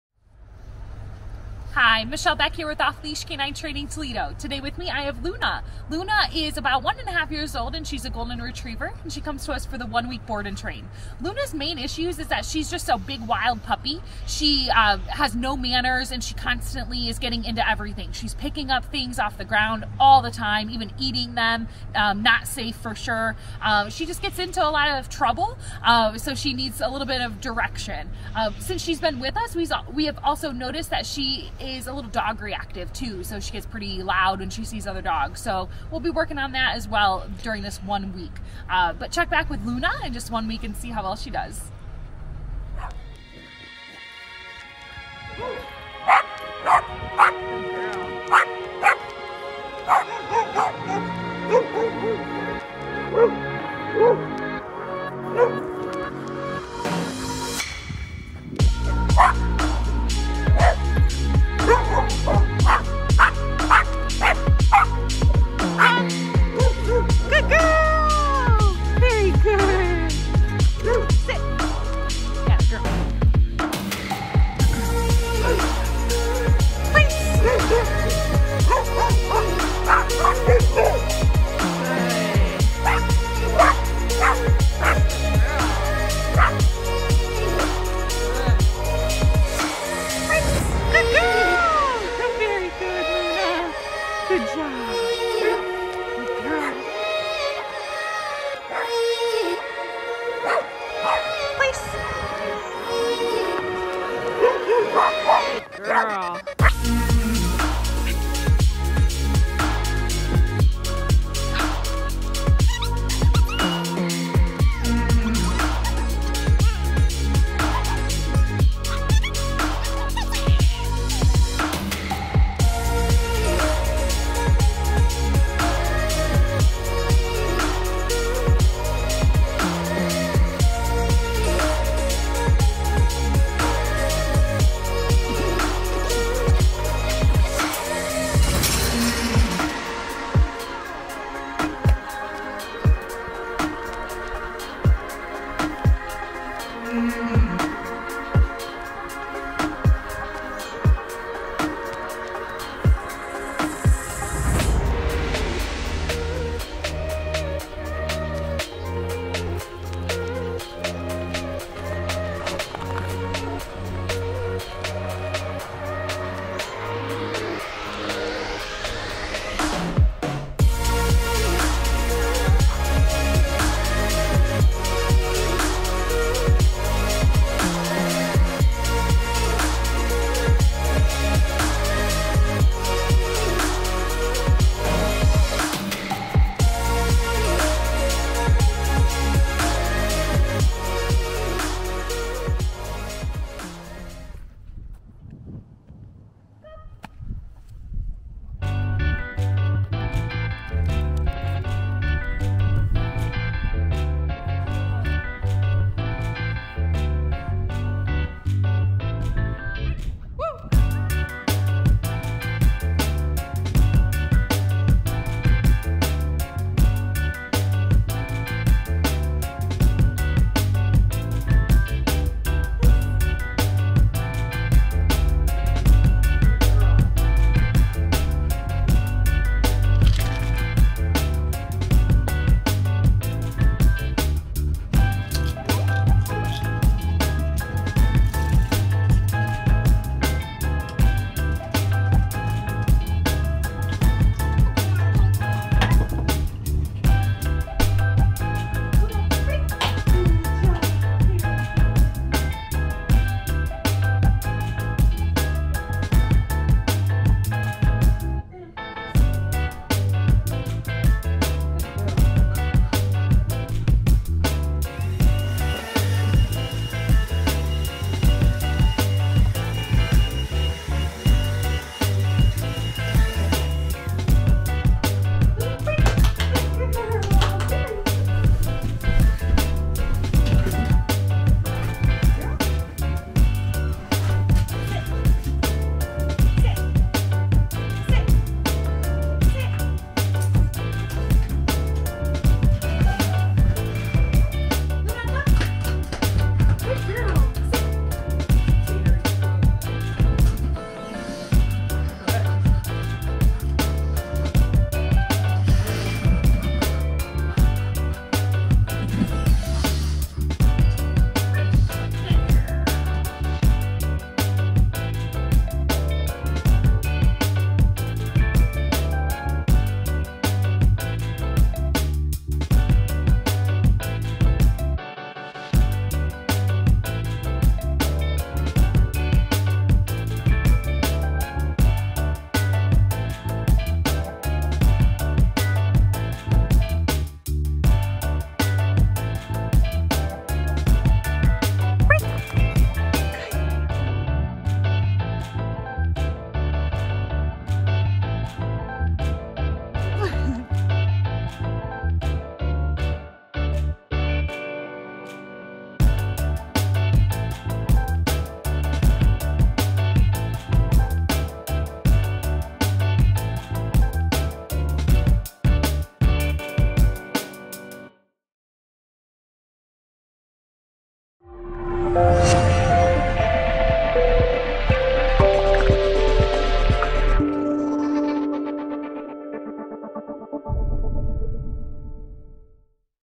Best Dog Training Toledo, Ohio! 2 Year Old Golden Retriever, Luna! One Week Board and Train
Take a look at the progress we were able to make with Luna in just 1 week!